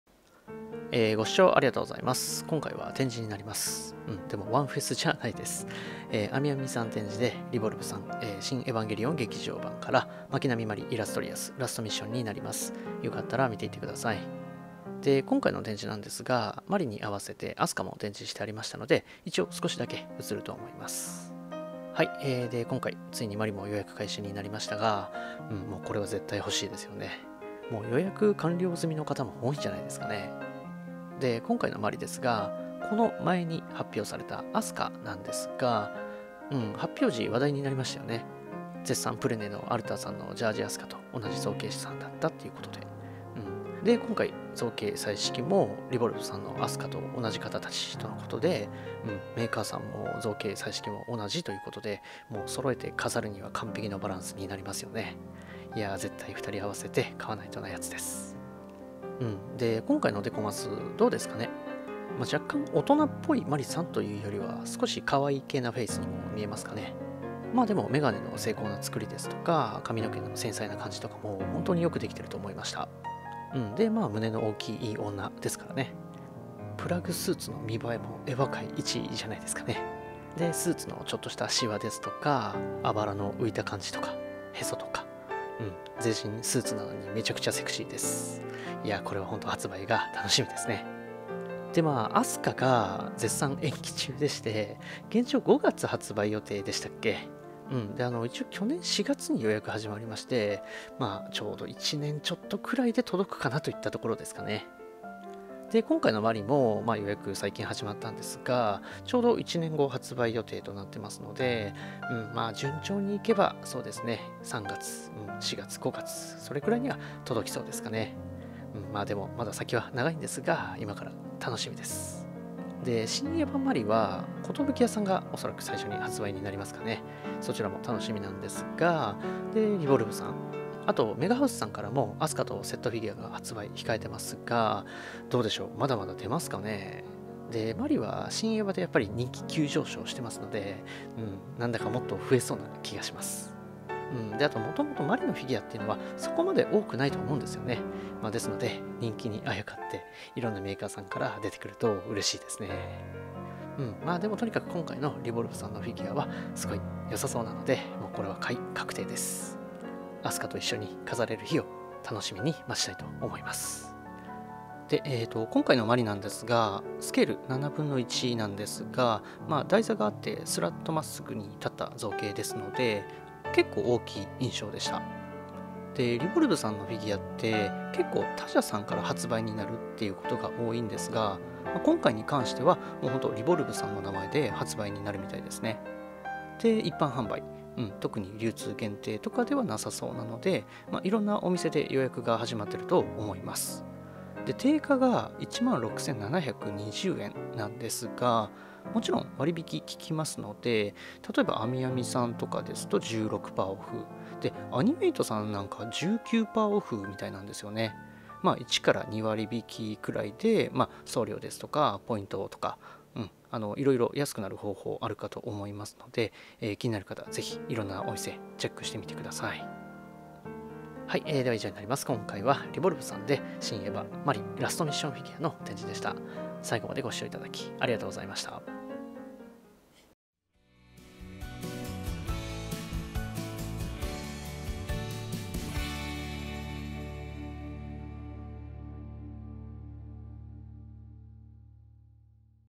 【展示】リボルブ シンエヴァ 真希波・マリ・イラストリアス [ラストミッション] フィギュア 【シン・エヴァンゲリオン劇場版】
Amazonページ
リボルブ シン・エヴァンゲリオン劇場版 真希波・マリ・イラストリアス ラストミッション 1/7スケール PVC ABS製 塗装済完成品フィギュア

リボルブ 真希波・マリ・イラストリアス［ラストミッション］

【作品名】シン・エヴァンゲリオン劇場版
【商品価格】定価16,720円（税込）

商品仕様
■商品形態：彩色済み完成品フィギュア
■サイズ：1/7スケール（全高約270mm）
■発売元：リボルブ
■原型制作：のぶた（リボルブ）
■彩色制作：かわも（リボルブ）